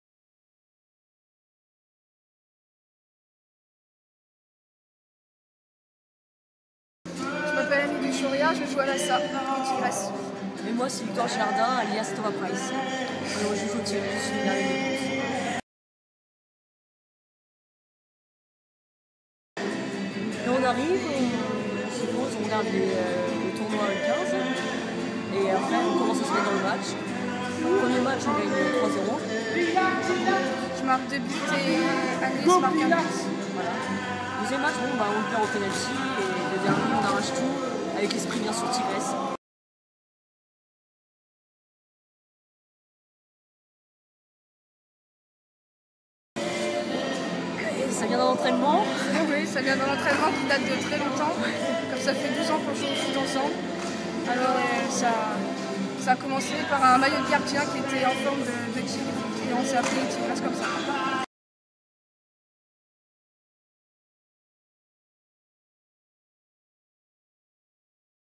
Les tigresses de l'AS Saint-Amand
Réactions des tigresses, gagnantes de la Coupe Régionale Futsal U19